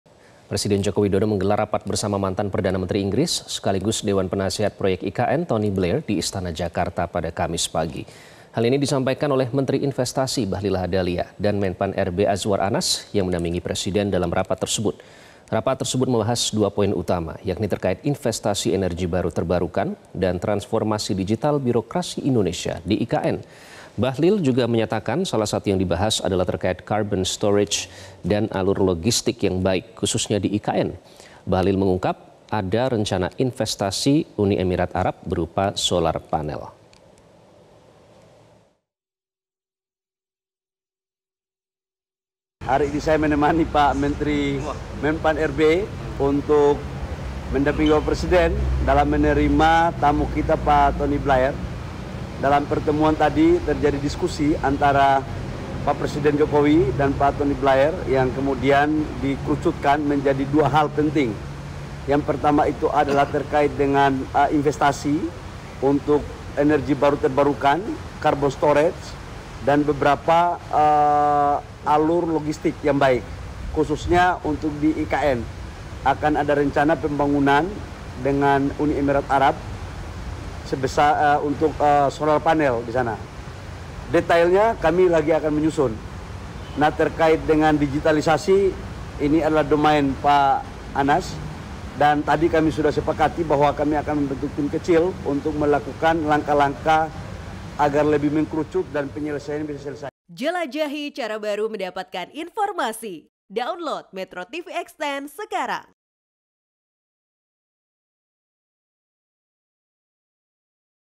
Jokowi & Tony Blair Bahas Rencana Proyek EBT di IKN
MetroTV, Presiden Joko Widodo menggelar rapat bersama mantan perdana menteri inggris sekaligus dewan penasihat proyek IKN Tony Blair di istana Jakarta, Kamis pagi (18/04/2024).